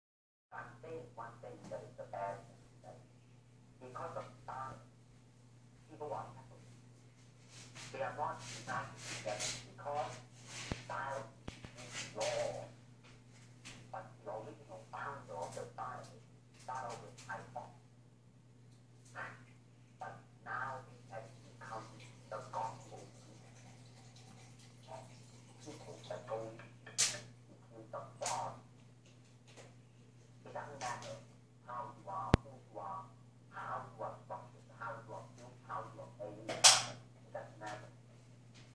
From Documentary video narrated by Bruce Lee: Tekki Shodan by Kenseikan Japan Team. See description:
This Classic Tekki Shodan was taped in April 1979 at a Public Demonstration in Tokyo Japan by our late Shihan Wataru Okawa 8th Dan. Later, this Video was included in a Bruce Lee's Documentary that was narrated by Bruce Lee himself. As per his comments, Bruce Lee is against having Karate styles which is rather unimportant for a true Karateka whose success is achieved through his/her own dedication and sacrifices combined with proper guidance-not just because of the style alone.
This solid Tekki Shodan Kata signifies the speed, and timing with a great focus performed by our late Shihan Dai Osanai, along with Shihan Dai Kiyochika Oka, (Chief Instructor), T. Sogo 3rd Dan, and few other Black Belts of Kenseikan Karate-Do.
In addition to this video another demo of Kenseikan Japan Dojo has been filmed and telecast by Canadian Television in May 1979 as part of a Japanese cultural show.